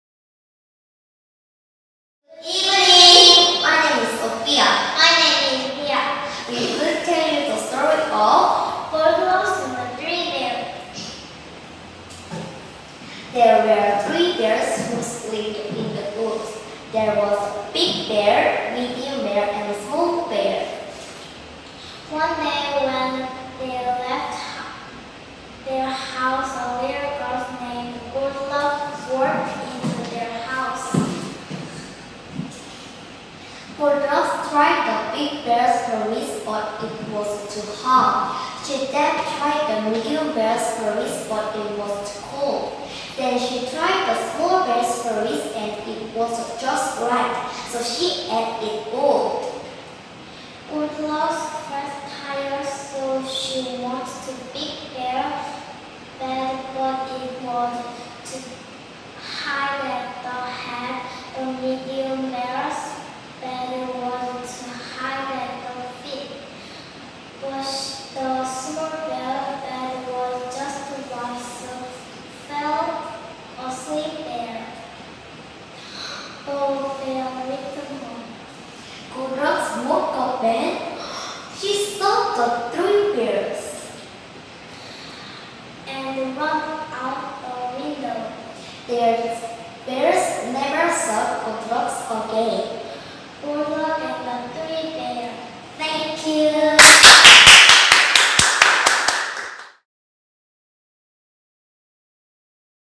Sophia and Bia's Story Telling (August 19, 2019)
Focus: pronouncing words with /l/ and /r/